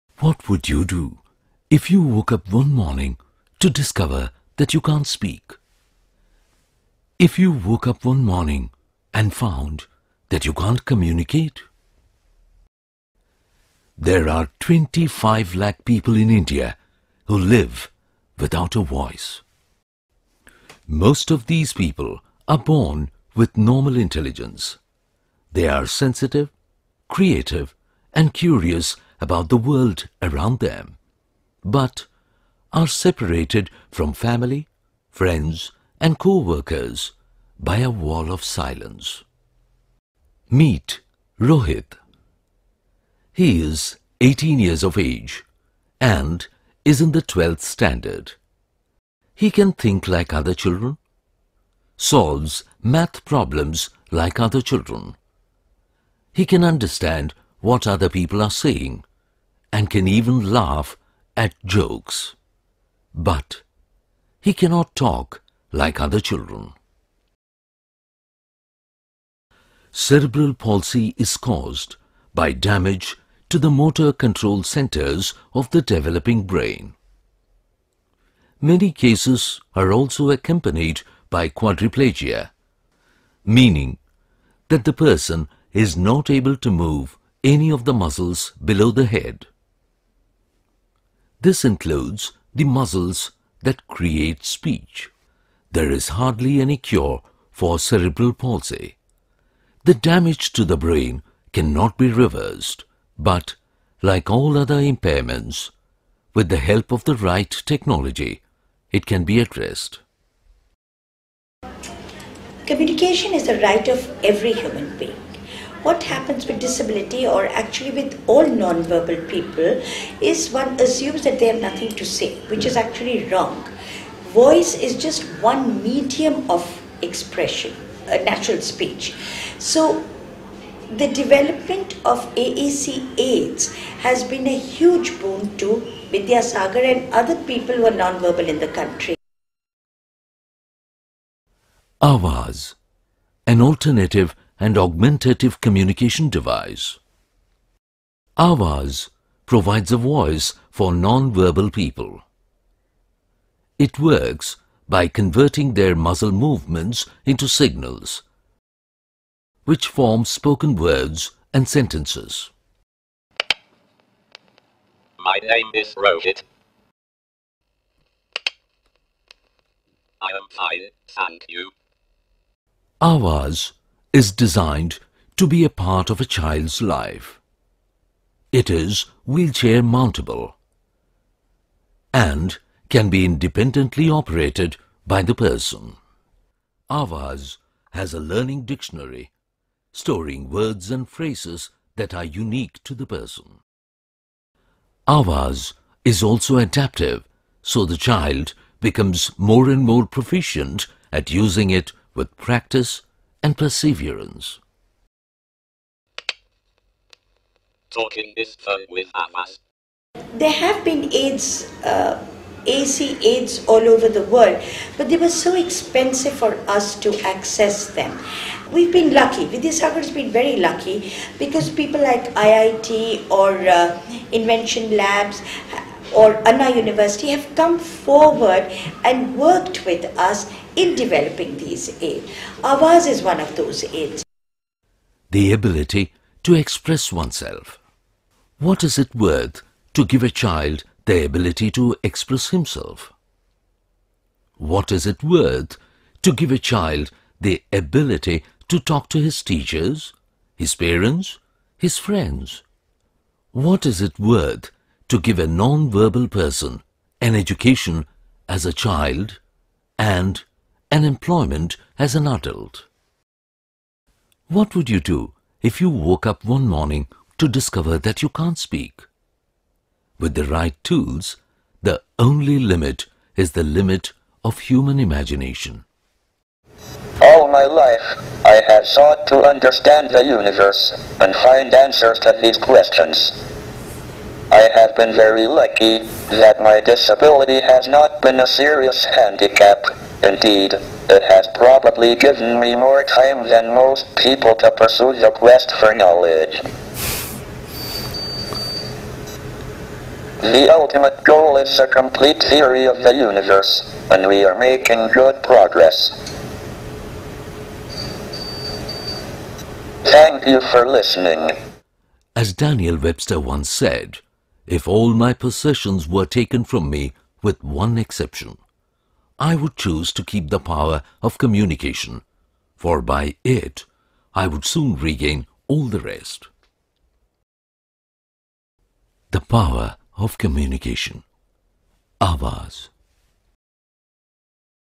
Augmentative and Alternative Commu-nication — AAC — device-AVAZ MASTER
Avaz is India's first commercially available Augmentative and Alternative Commu-nication — AAC — device. It enables people with disabilities like cerebral pal-sy, autism, mental retardation and speech disorders to communicate. Avaz converts limited muscle movements into speech.